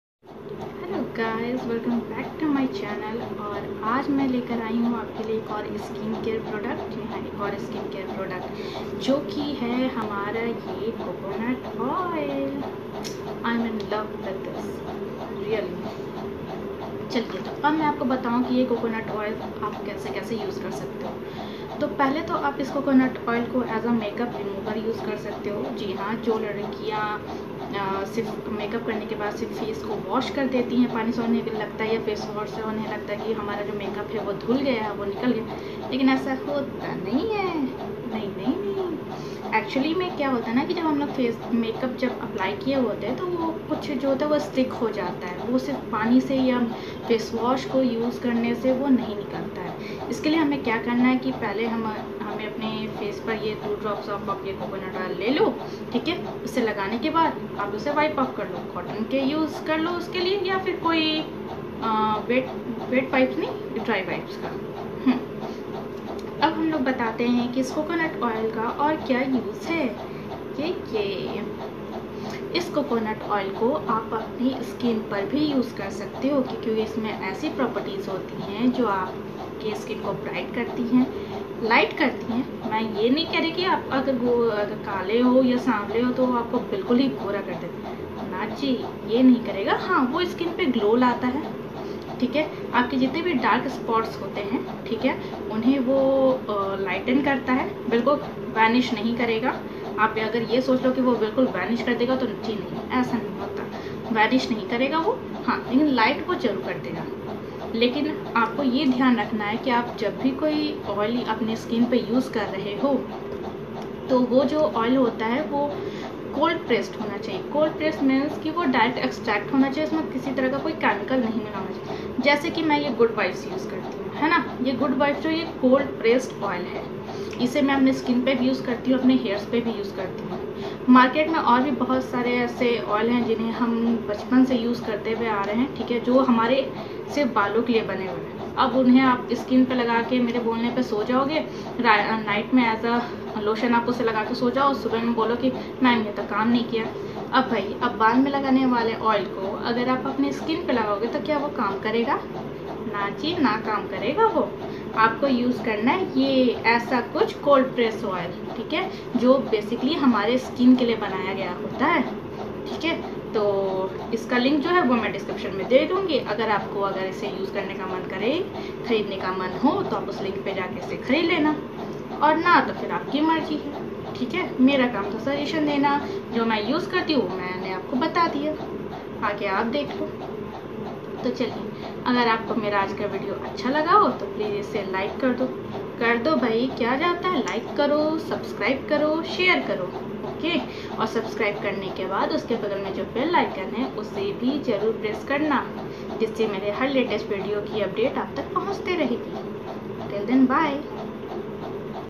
COCONUT OIL FOR BEAUTIFUL SKIN / 2018 ||Divya
Today's video is all about coconutoil..uses coconutoil different ways.

Credit -
Good vibes Coconut oil